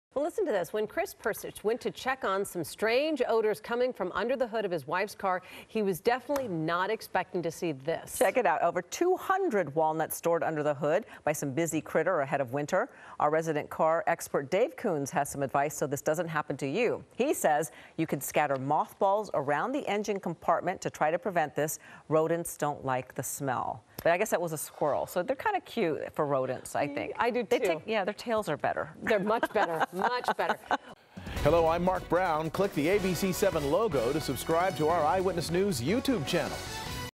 THAT'S NUTS! Pittsburgh woman finds squirrel's winter stash of nuts under car hood | ABC7
The car owner smelled something she thought was burning gas, and popped the hood to discover the squirrel's stash of over 200 walnuts.